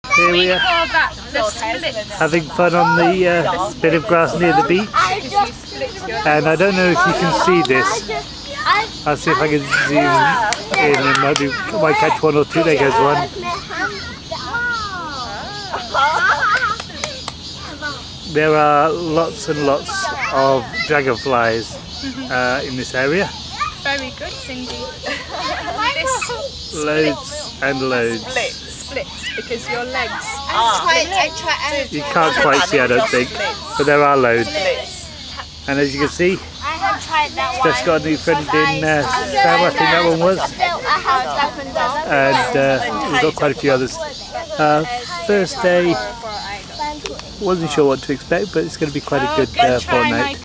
Dragonflies at the beach in Dalian, China
Catching dragonflies at the beach in Dalian